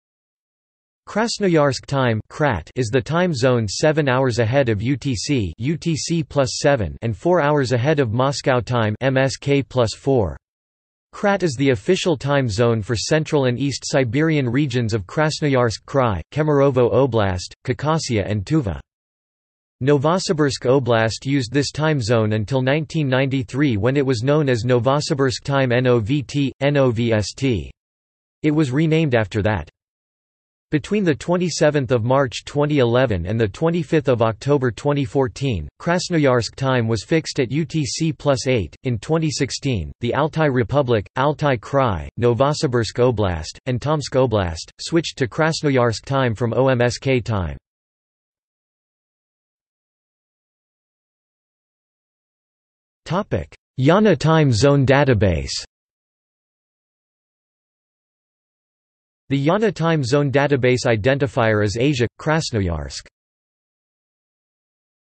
Krasnoyarsk Time | Wikipedia audio article
This is an audio version of the Wikipedia Article:
Krasnoyarsk Time

00:00:49 undefined

- Socrates

SUMMARY
Krasnoyarsk Time (KRAT) is the time zone seven hours ahead of UTC (UTC+7) and 4 hours ahead of Moscow Time (MSK+4).  KRAT is the official time zone for central and east Siberian regions of Krasnoyarsk Krai, Kemerovo Oblast, Khakassia and Tuva.
Novosibirsk Oblast used this time zone until 1993 when it was known as Novosibirsk Time NOVT/NOVST. It was renamed after that.
Between 27 March 2011 and 25 October 2014, Krasnoyarsk Time was fixed at UTC+8.In 2016, the Altai Republic, Altai Krai, Novosibirsk Oblast, and Tomsk Oblast, switched to Krasnoyarsk Time from Omsk Time.